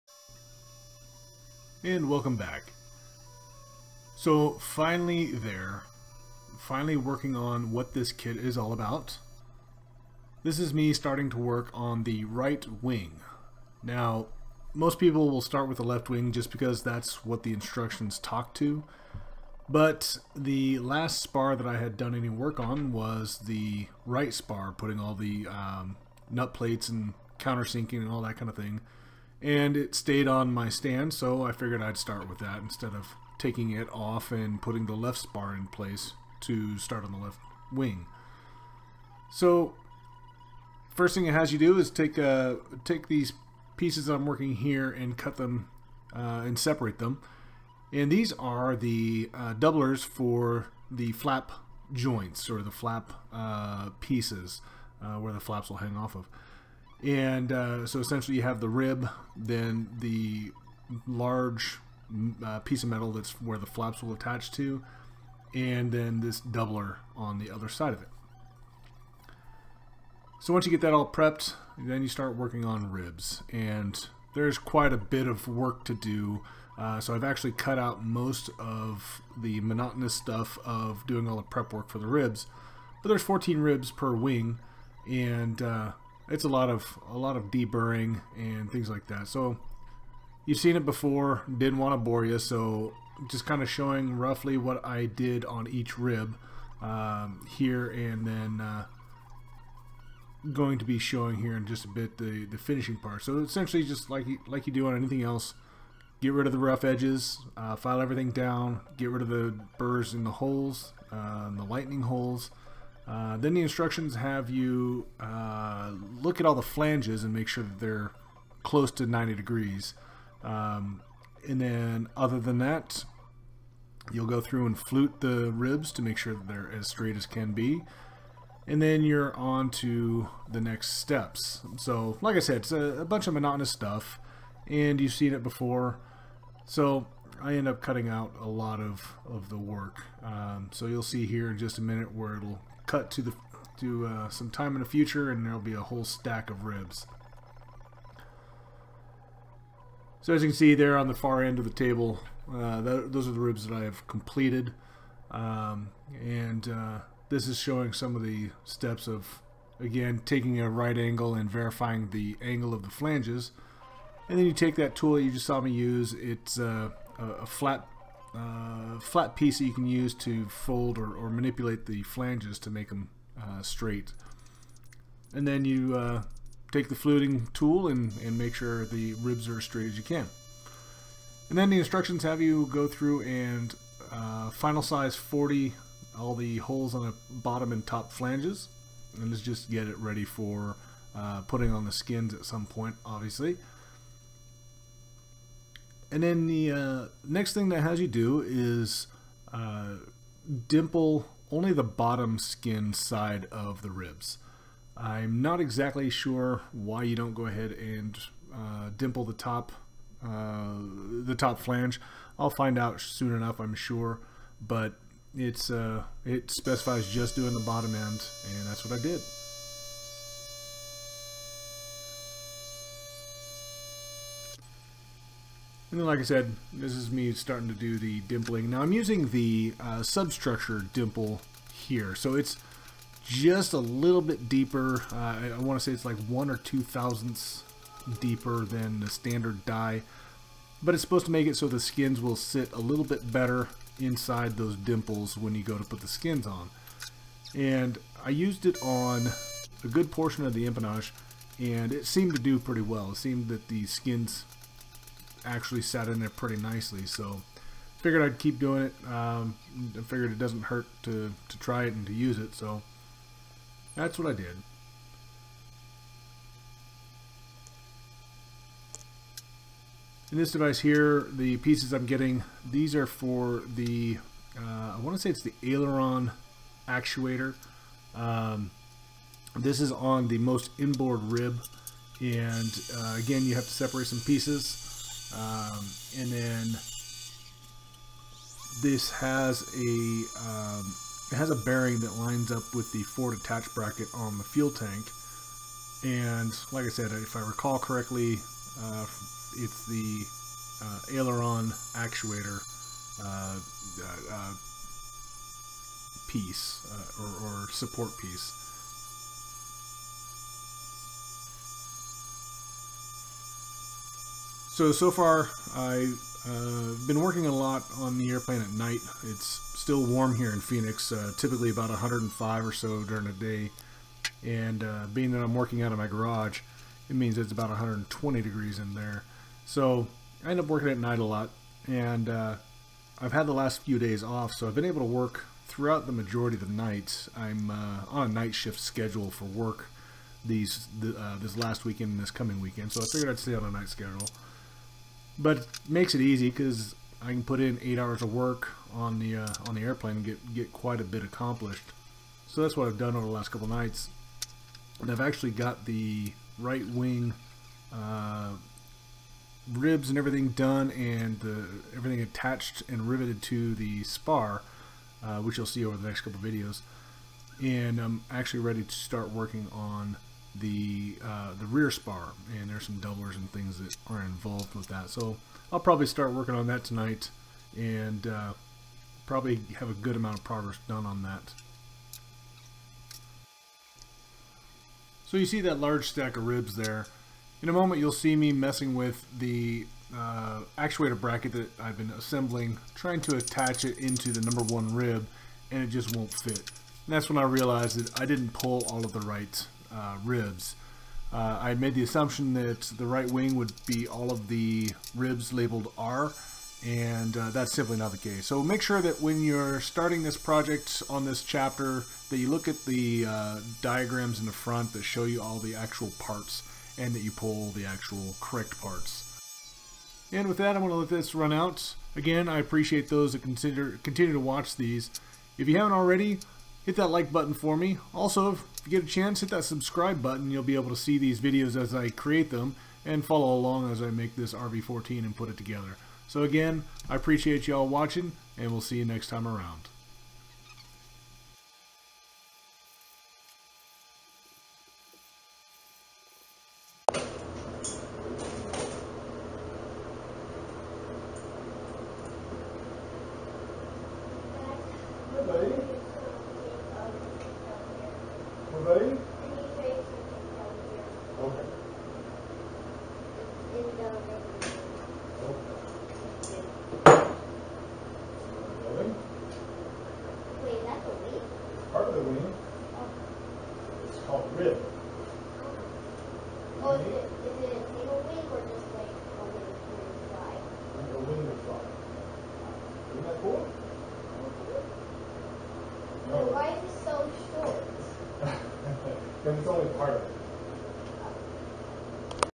Scott's RV 14a Right wing part 1
Finally starting to make something that looks similar to a wing! In this video, I discuss the prep work involved with the right wing along with some of the "gotchas" that I encountered.

Hint- read EVERYTHING!